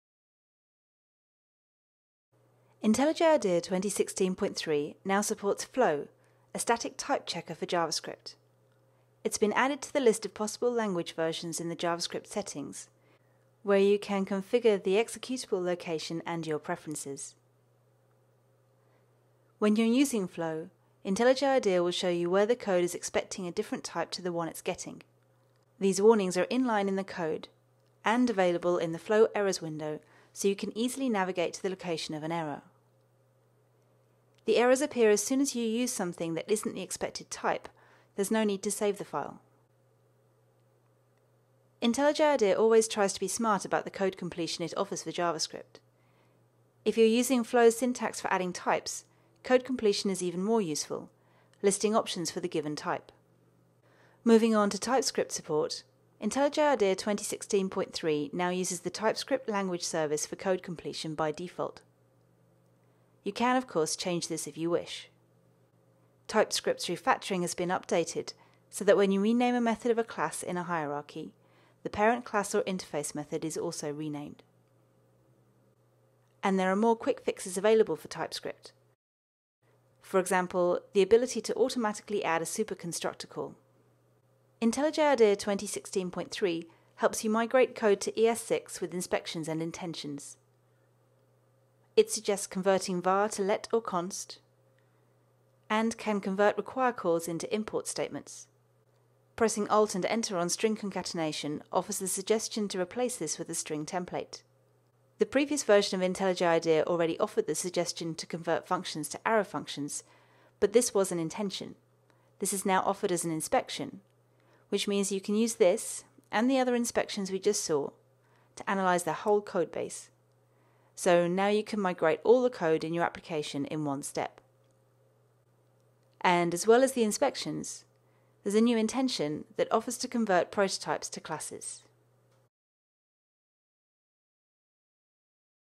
JavaScript in IntelliJ IDEA 2016.3
- Support for Flow
 - Improved TypeScript support
 - Additional inspections for migrating to ES6